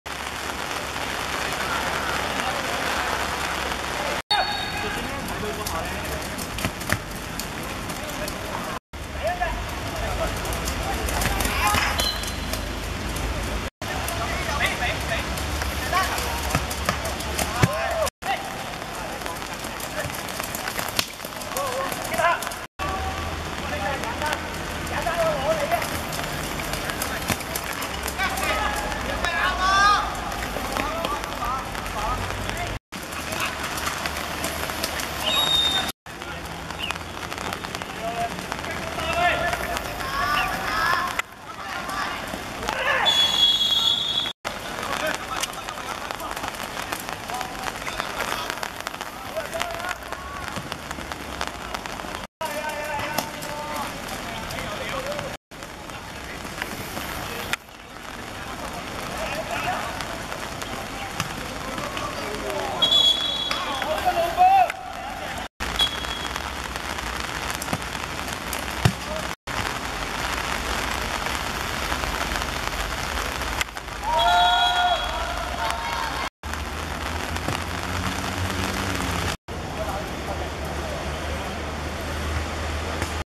House One vs 左羊右華(2015.7.23.青協盃公開組分組賽)精華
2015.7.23.觀塘康寧道球塲 12:00 House One(黑) 2:1 左羊右華(白)